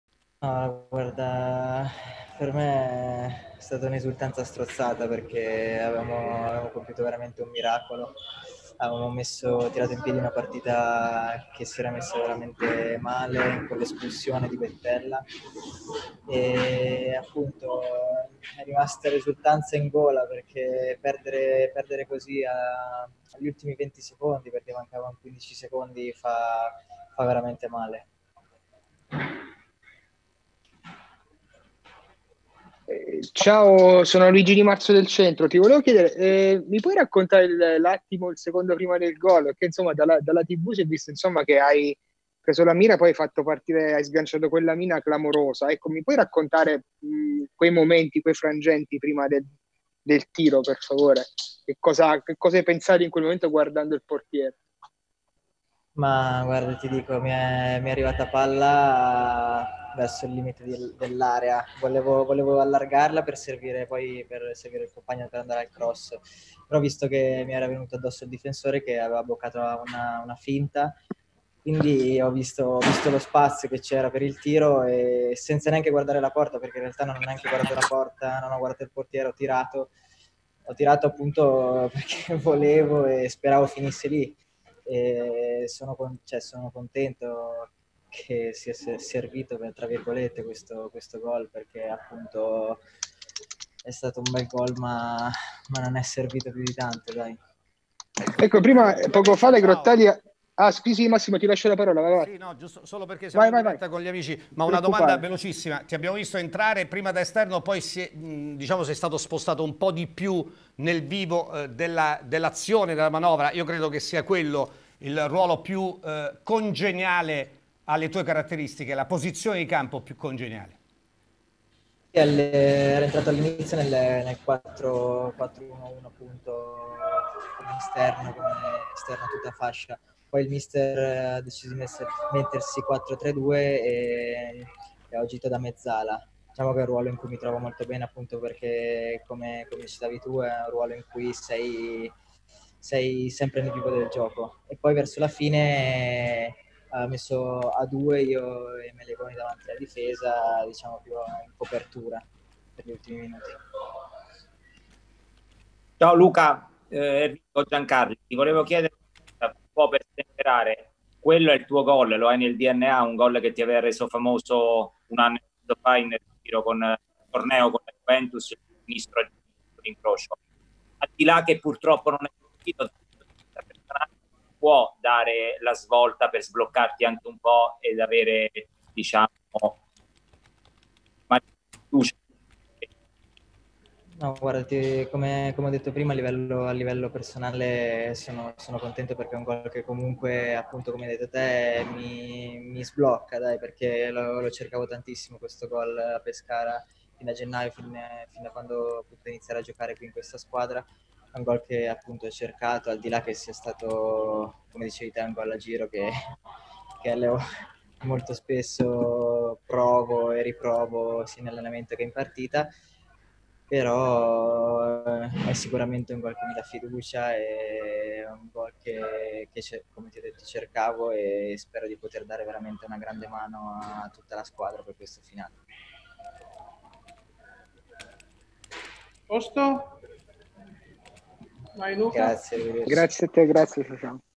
Pisa - Pescara 2-1: Luca Clemenza
Conferenza post gara dell'autore del gol del momentaneo pareggio del Pescara, il primo con la maglia biancazzurra